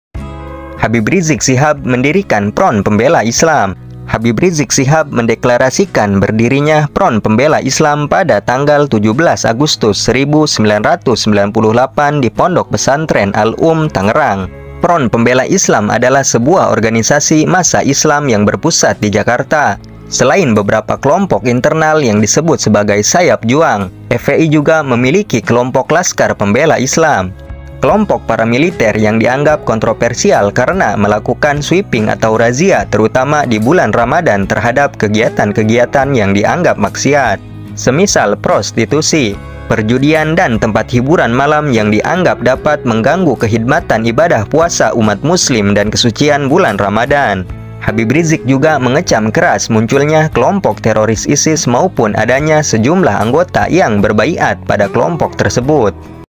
Habib Rizieq Shihab Mendirikan Front Pembela Islam - Biografi Habib Rizieq Shihab Part 4
HABIB RIZIEQ SHIHAB MENDIRIKAN FRONT PEMBELA ISLAM
Habib Rizieq Shihab mendeklarasikan berdirinya Front Pembela Islam pada tanggal 17 Agustus 1998 di Pondok Pesantren Al-Umm, Tangerang.
Front Pembela Islam adalah sebuah organisasi massa Islam yang berpusat di Jakarta.
Selain beberapa kelompok internal yang disebut sebagai Sayap Juang, FPI juga memiliki kelompok Laskar Pembela Islam, kelompok paramiliter yang dianggap kontroversial karena melakukan sweeping atau razia terutama di bulan Ramadan terhadap kegiatan-kegiatan yang dianggap maksiat. semisal prostitusi, perjudian, dan tempat hiburan malam yang dianggap dapat mengganggu kehidmatan ibadah puasa umat muslim dan kesucian bulan Ramadan.
Habib Rizieq juga mengecam keras munculnya kelompok teroris ISIS maupun adanya sejumlah anggota yang berbaiat pada kelompok tersebut.